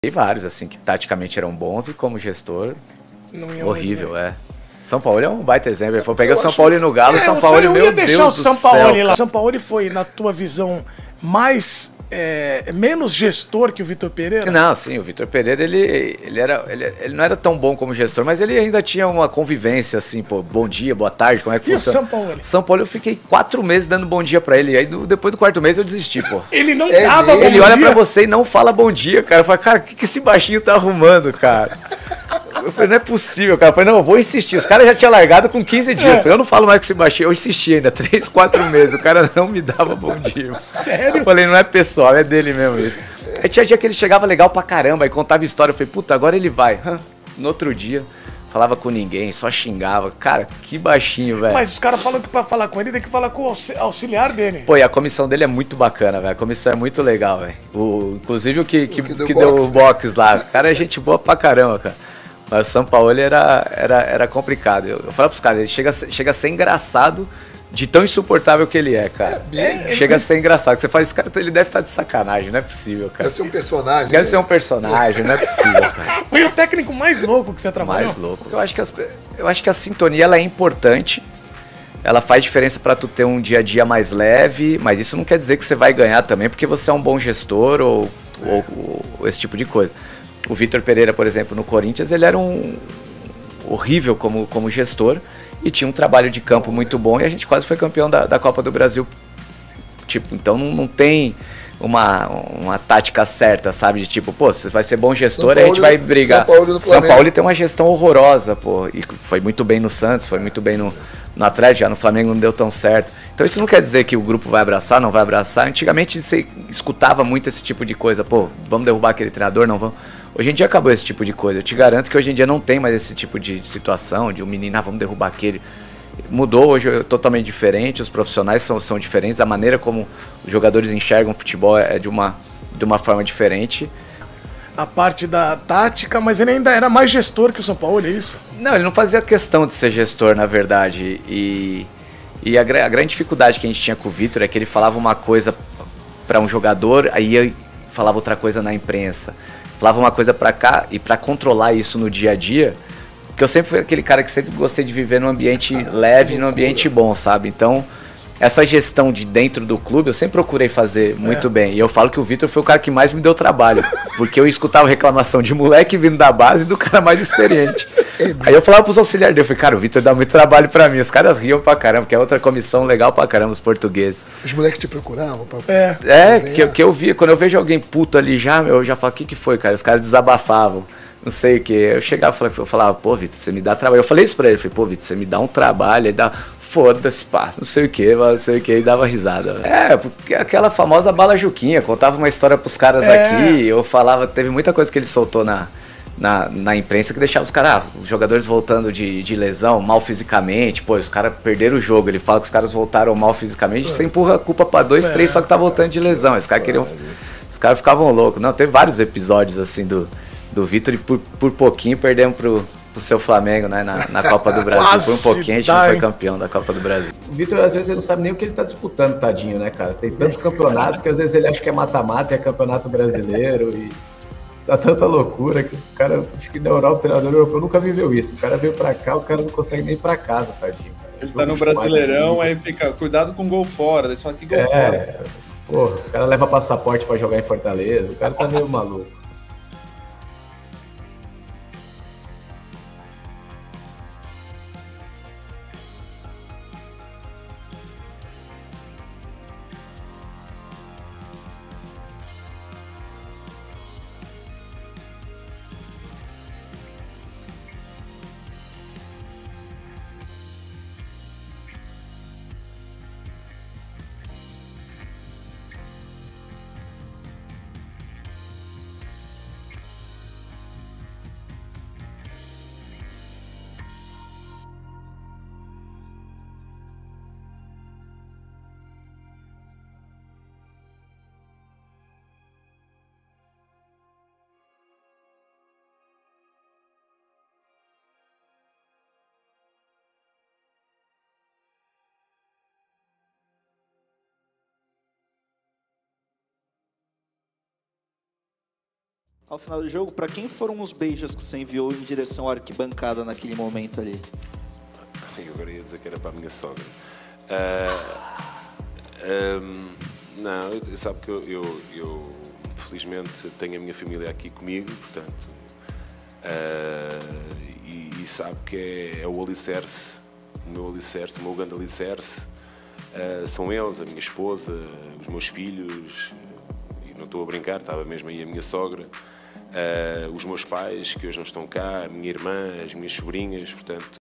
TRETA ABSURDA! VlT0R PERElRA E FABl0 SANTOS SE ENVOLVEM EM P0LÊMICA APÓS CAGUETAR SUPOSTA TRAIRAGEM!
O embate fervilhante entre Fabio Santos e Vitor Pereira foi como um vulcão em erupção, onde as palavras se transformaram em lava incandescente. Fabio, traído pela traição de Vitor, não hesitou em expor a traição desleal, desencadeando uma avalanche de acusações. Os gritos ecoavam como trovões em uma tempestade, cada palavra uma punhalada profunda no vínculo outrora sólido entre eles. O ar ficou pesado com a tensão, enquanto cada um defendia sua versão da traição, criando um clima de hostilidade irremediável.